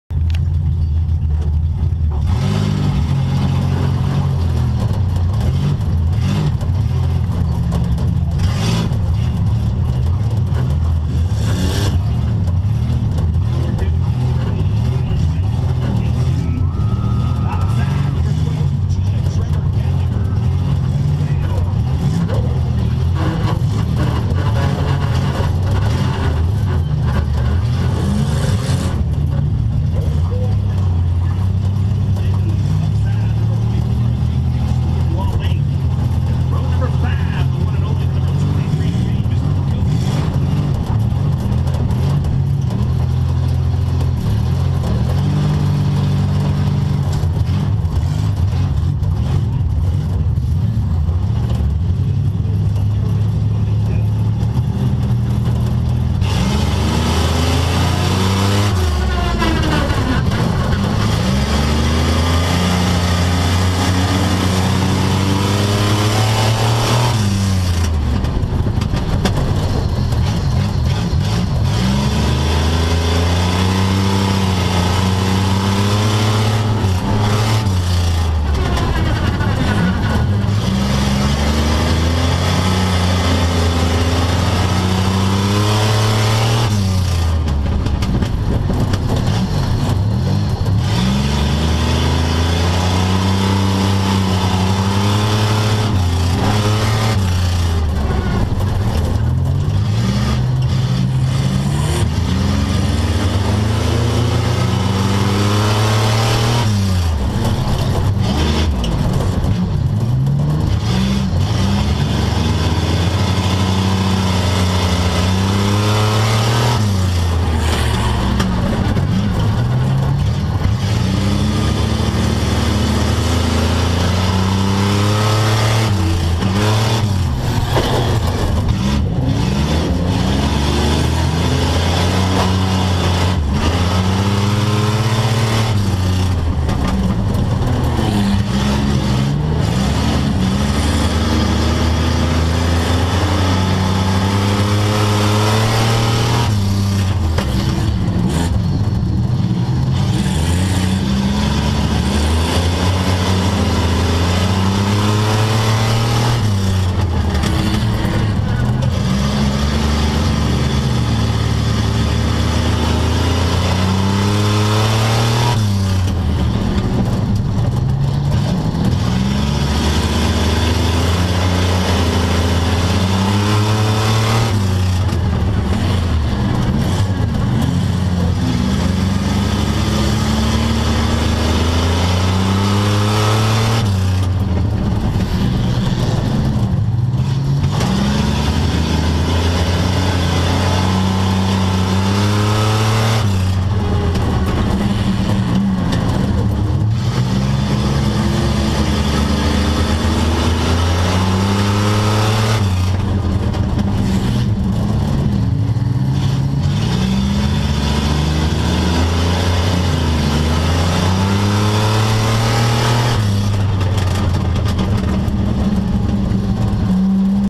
Audubon Figure 8 6/26/20 FWD Stock Heat in car
Got 2nd place to advance to b feature